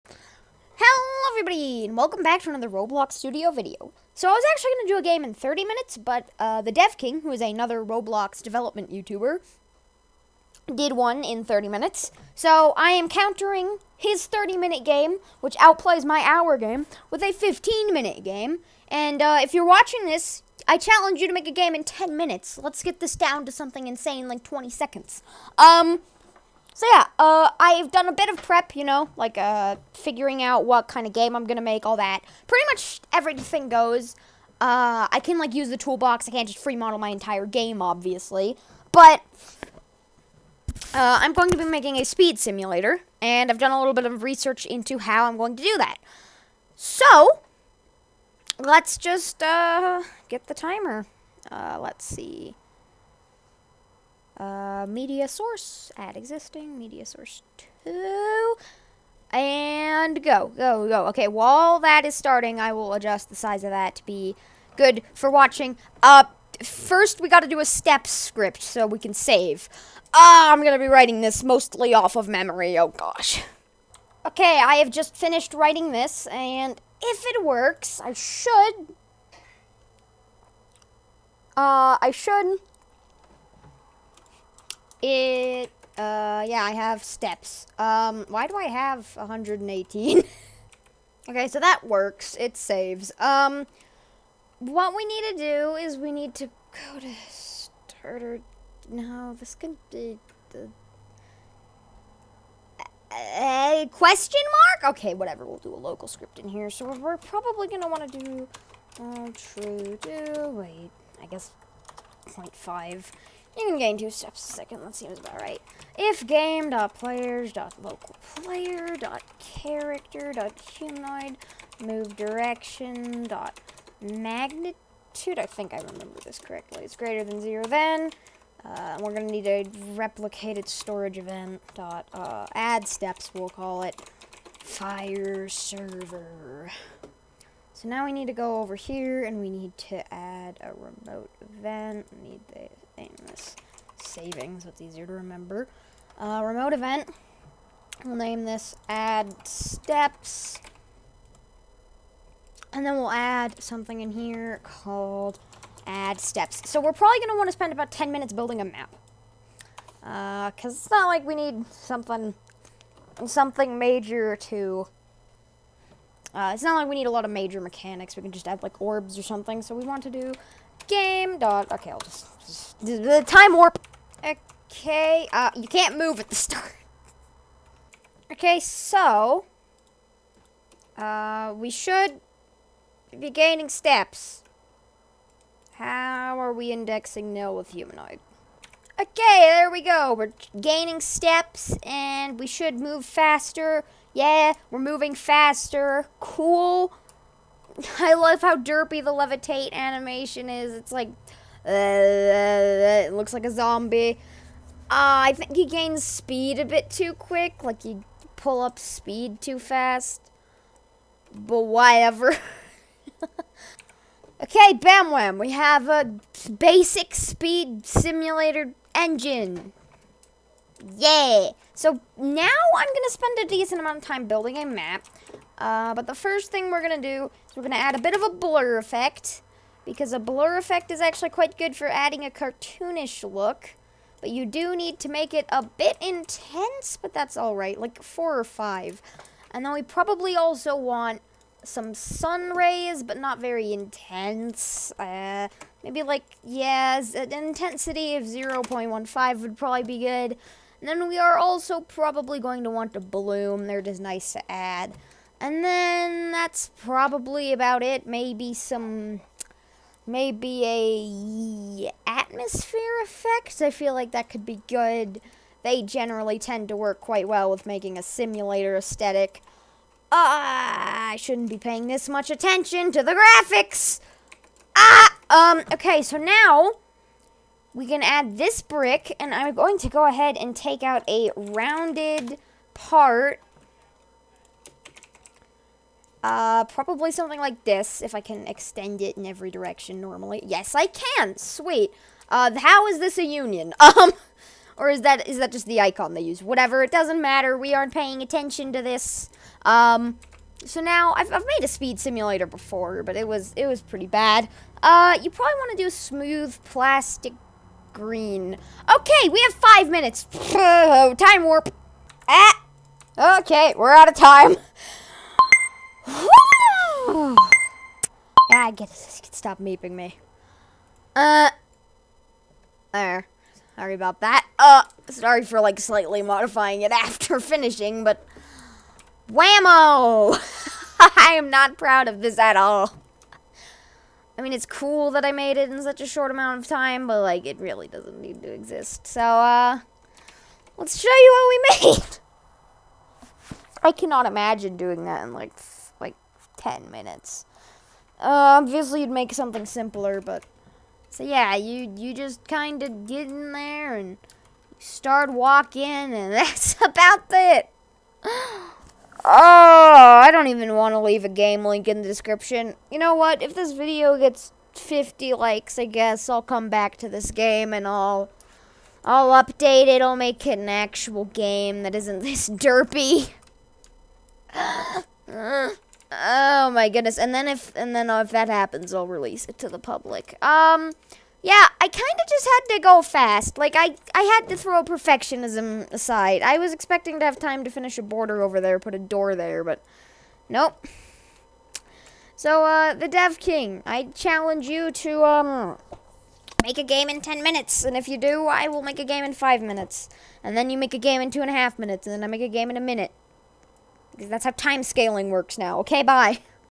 I Made A Game In 15 Minutes Because @TheDevKing Did It In 30 Minutes (Roblox Studio)
This was tons of fun and I'd love to keep doing stuff like this, so, dev king, if you're reading this, I challenge you to make a game in 10 minutes!